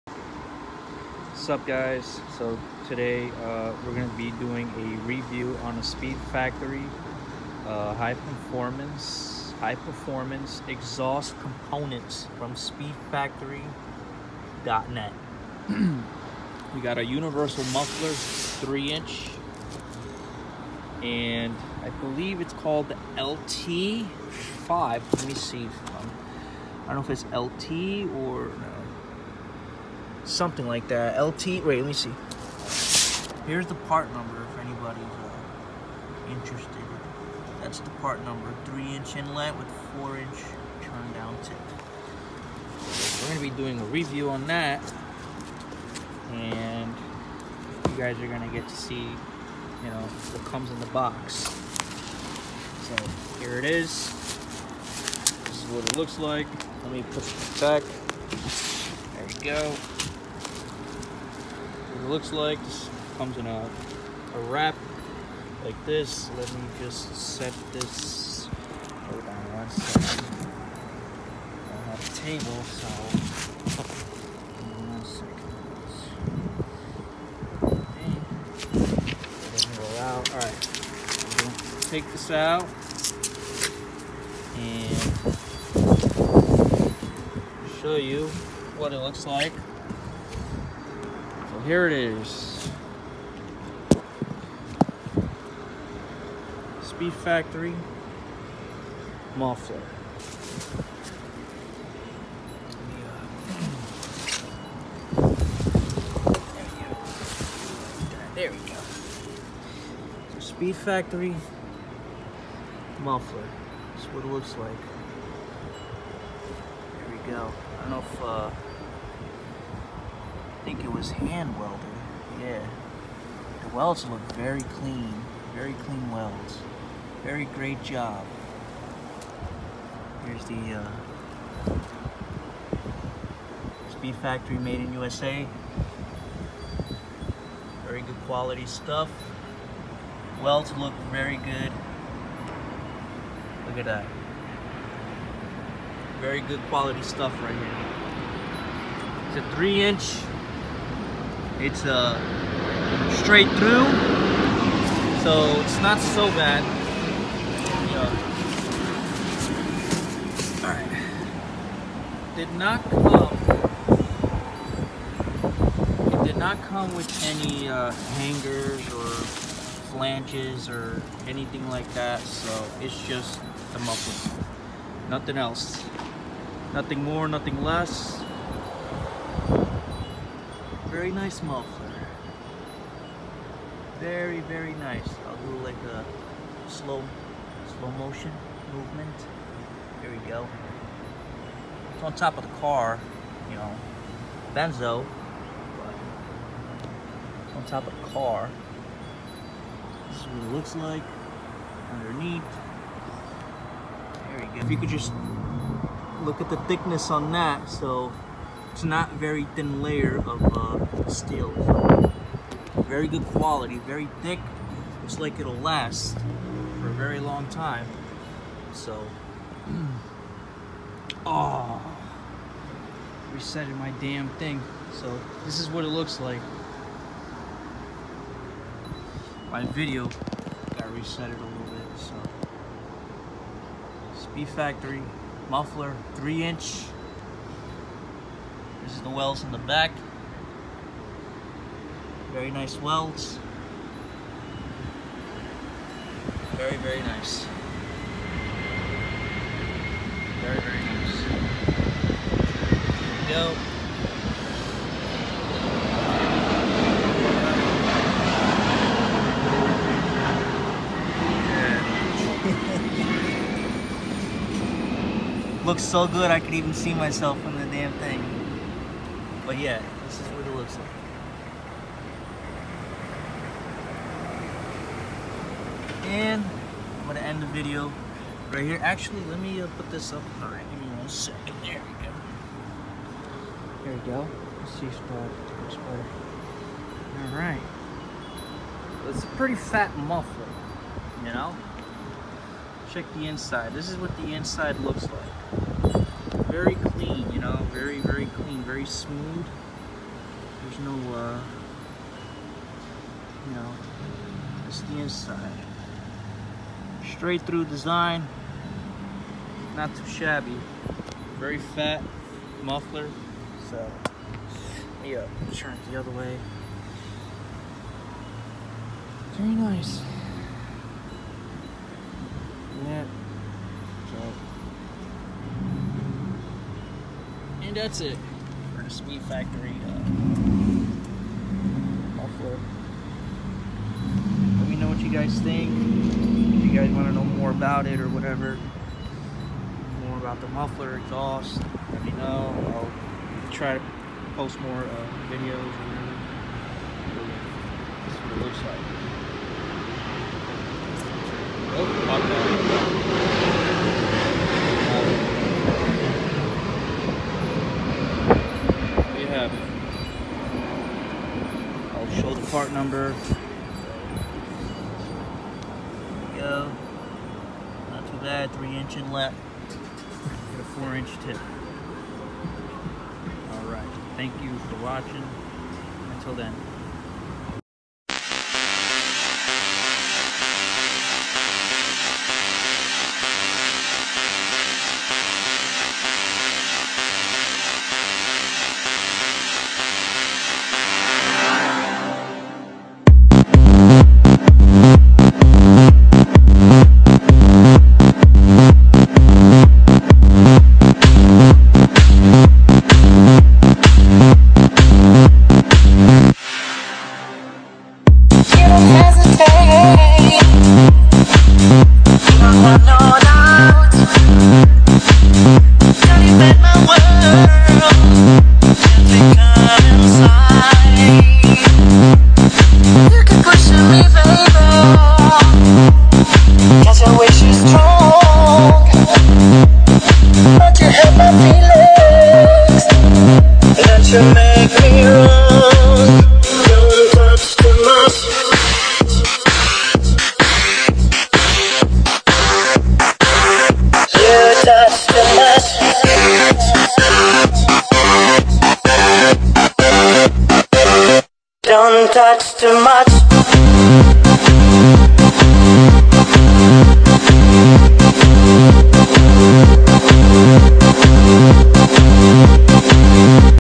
SpeedFactory Muffler LT-5 Race Muffler 3inch Review
SpeedFactory Lt-5 Race Muffler
Not a Solid Review but more of like an observation showing of the Muffler it self!
3inch Inlet Diameter
4inch tip

Available sizes are from 2.5 to 3.5

Muffler Body Only: 13.75" Length x 6.0" Diameter
Muffler Body with Inlet: 16.0" Length
Tip: 4.0” Diameter, 5pc pie cut turndown
8lbs weight

Part number  SF-04-100